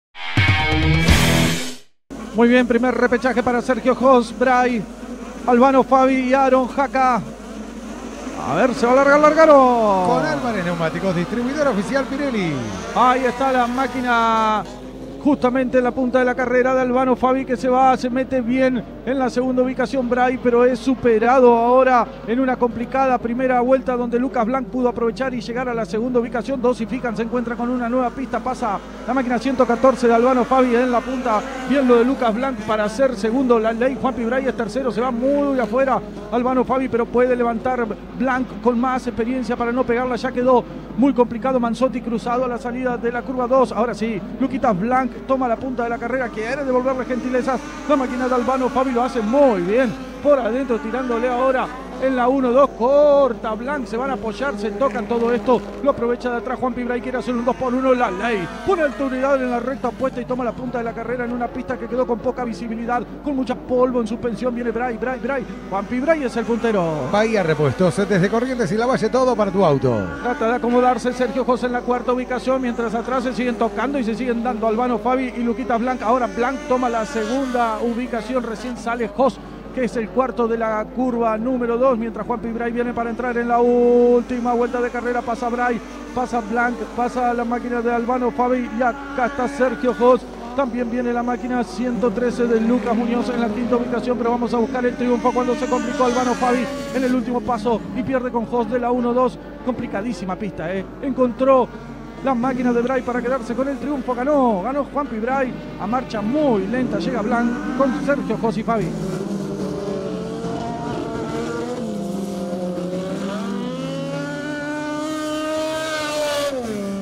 2024 - Fecha 2 - Repechaje 1 GRUPO 2
Campeonato Invernal del Midget 2024 - 2da Fecha Grupo 2
Imágenes: A Las Chapas TV
Relatos: A Las Chapas Radio, LU2 Radio Bahía Blanca (AM 840 y FM 92.7)